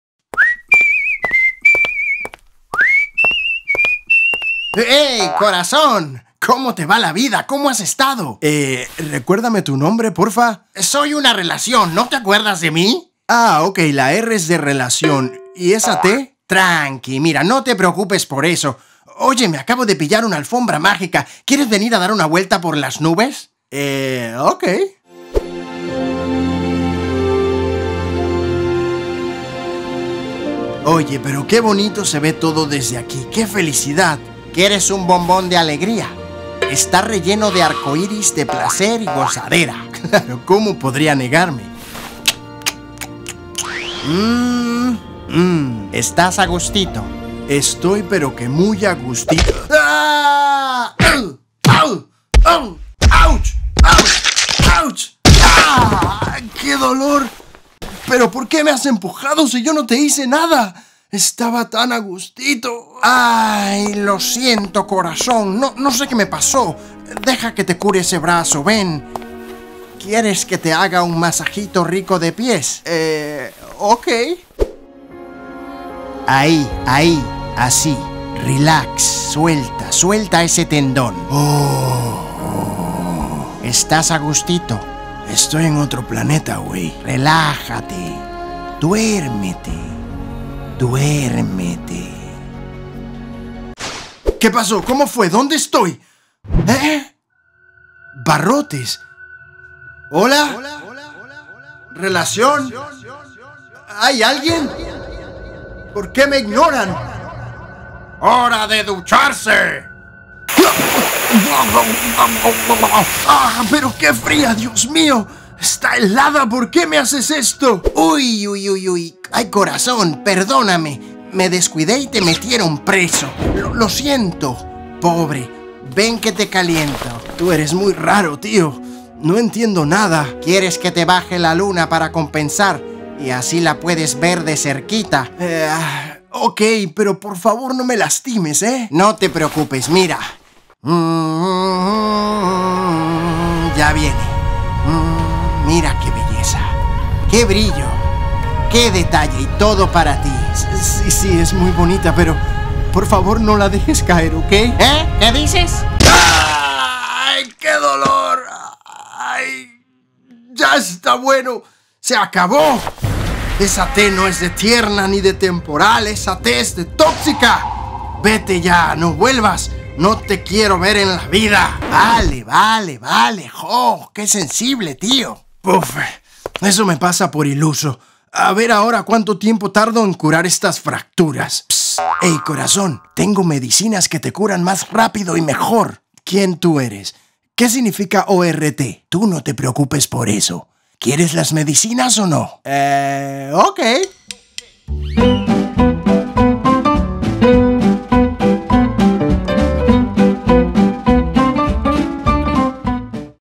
Tu corazón cuando estás en una relación tóxica | Casi Creativo
Tu corazón cuando estás en una de esas relaciones tóxicas. Una de esas montañas rusas con triples vueltas y bajadas abismales. Uno de esos amores que te baja la luna y al rato te la tira encima.
Casi Creativo, canal de vídeos animados, algunos entretenidos otros interesantes.
Vídeo nuevo cada semana. Like, comenta y comparte!